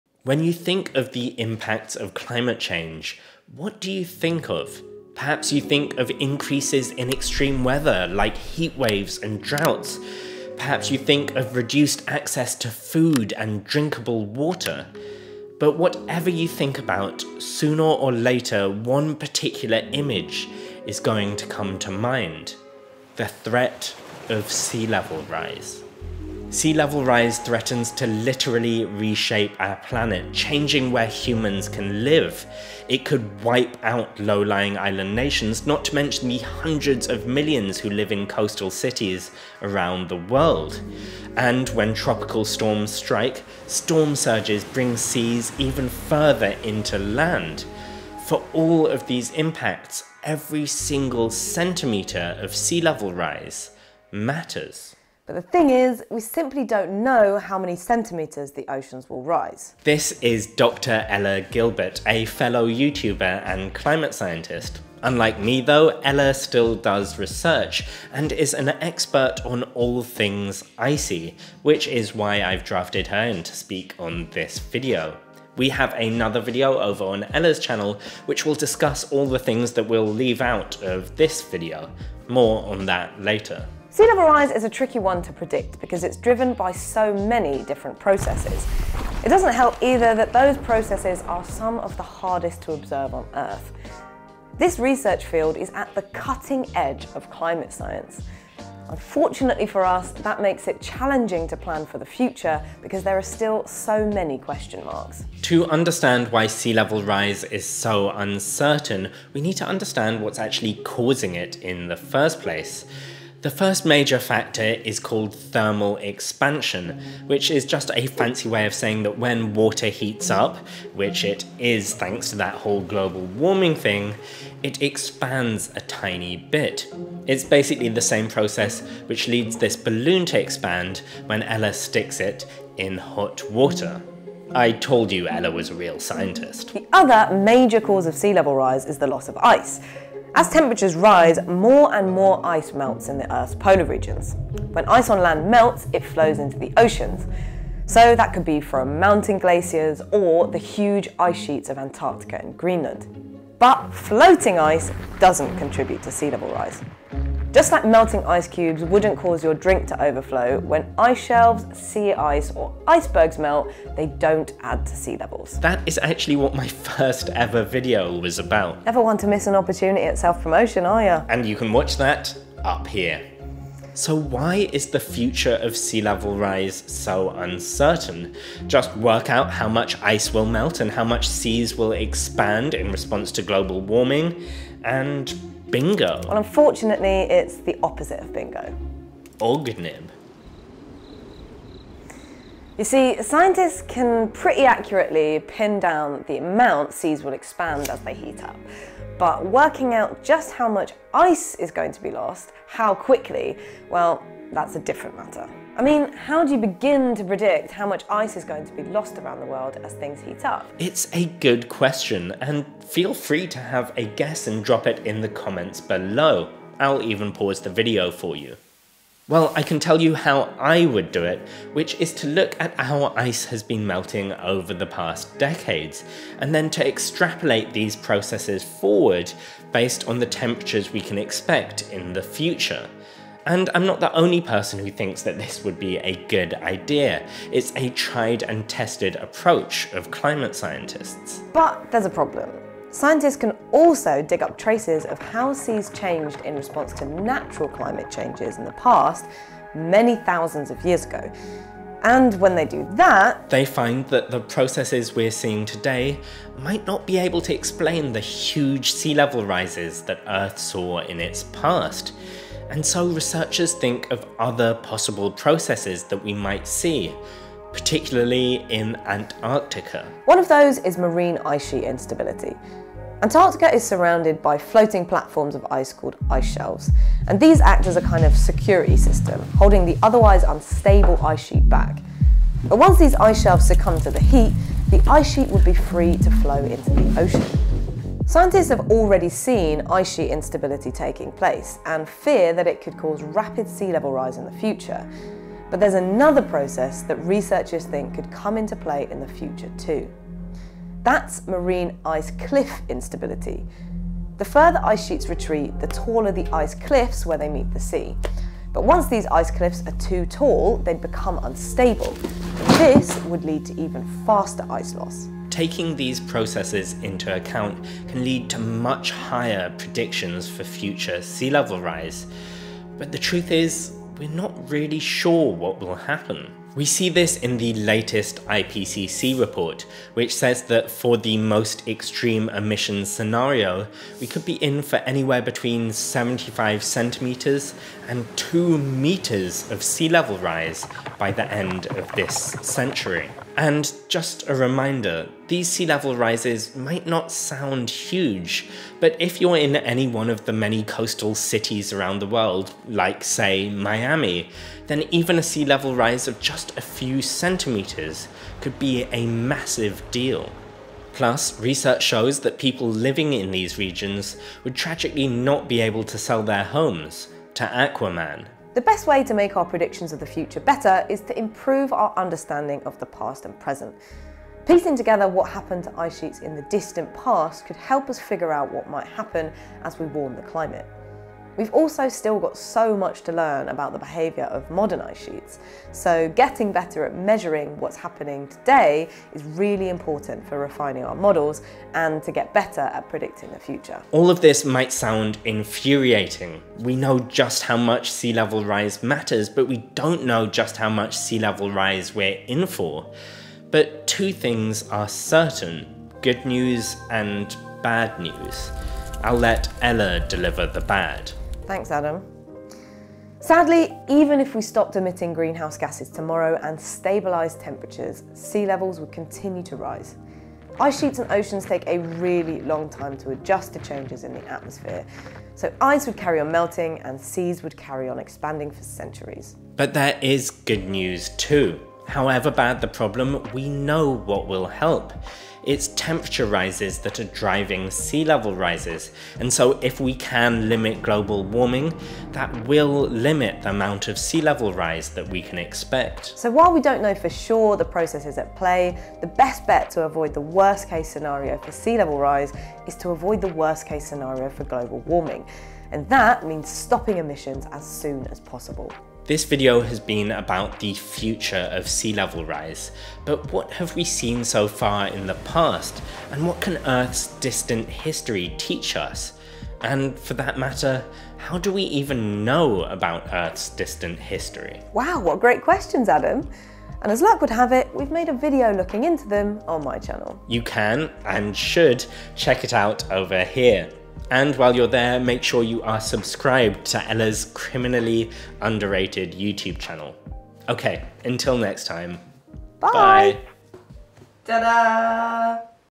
How high will sea levels rise?? [with @DrGilbz]
What will the Sea Level Rise of tomorrow look like? The truth is there's still a big range of possibilities, and scientists are struggling to pin down the processes that come into play. But one thing is clear: to stay safe, we need to stop emitting as soon as possible.

==MORE INFO==

==THANKS==

Thanks to Ella Gilbert of course
Also thanks to Alex Paulin Booth for directing
Storm surge photo by Krystal Hamlin